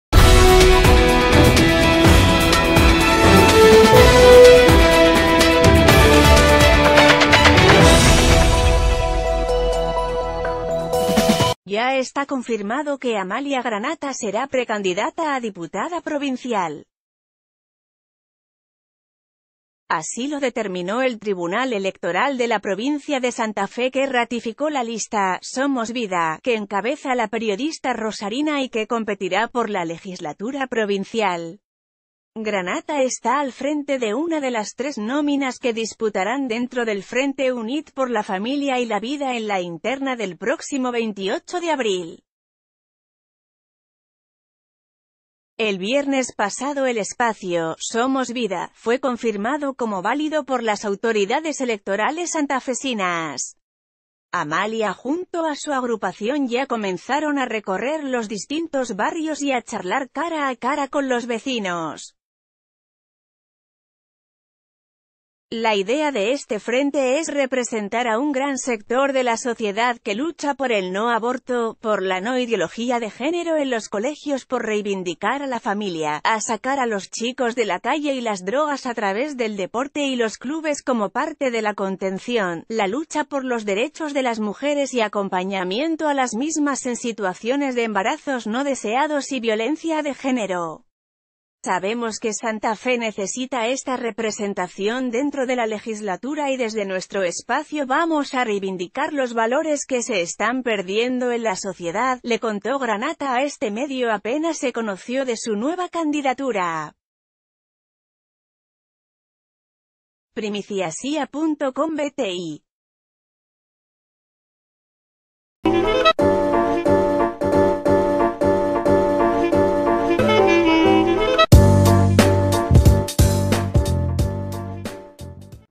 El Tribunal Electoral de Santa Fe ratificó la lista que encabeza Amalia Granata
Ya está confirmado que Amalia Granata será precandidata a diputada provincial. Así lo determinó el Tribunal Electoral de la provincia de Santa Feque ratificó la lista “Somos Vida” que encabeza la periodista rosarina y que competirá por la Legislatura provincial. Granata está al frente de una de las ...